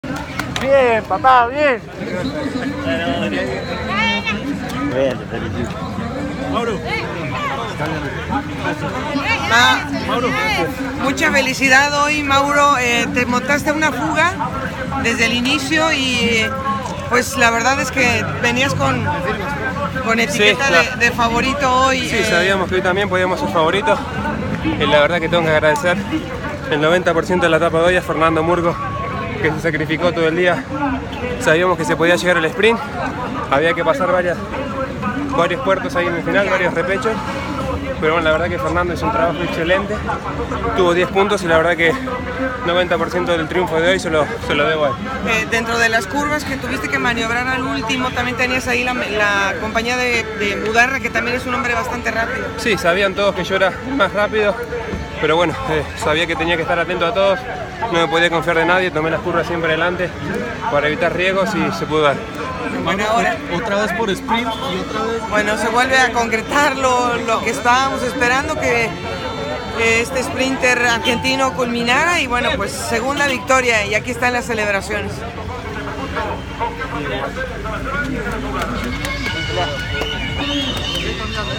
Mauro Richeze saca segunda victoria en Vuelta CRC 2015
El sprinter argentino Mauro Richeze (San Luis Somos Todos) venció en la etapa 7 y acumuló dos consecutivas VuelraCR